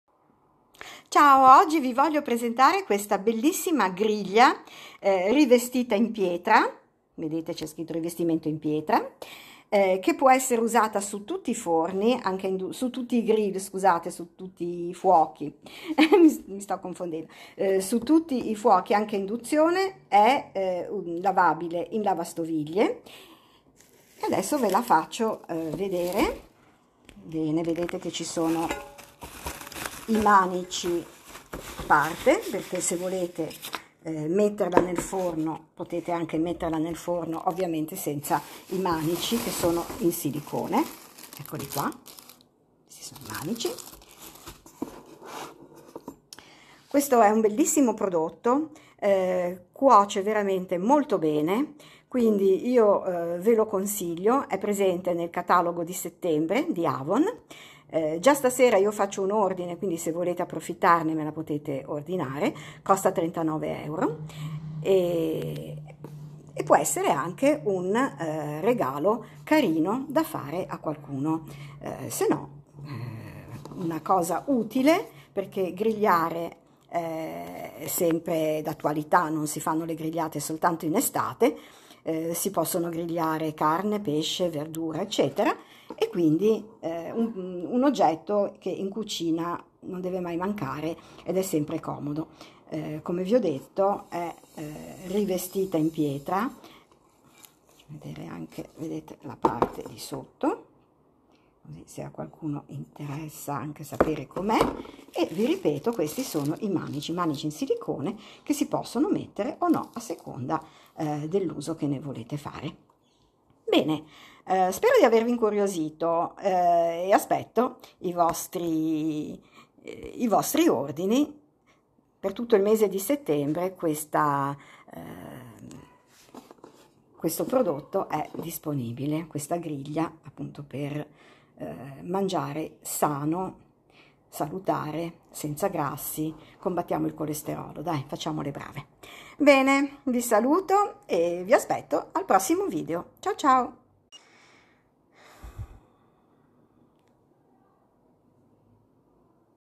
Grigliate non solo in estate
Griglia in alluminio pressato rivestita in pietra per cucinare qualsiasi cibo senza bisogno di grassi. Adatto a tutti i tipi di fuochi,  anche induzione. Lavabile in lavastoviglie
Lo trovi sul mio catalogo Avon  on line del mese di settembre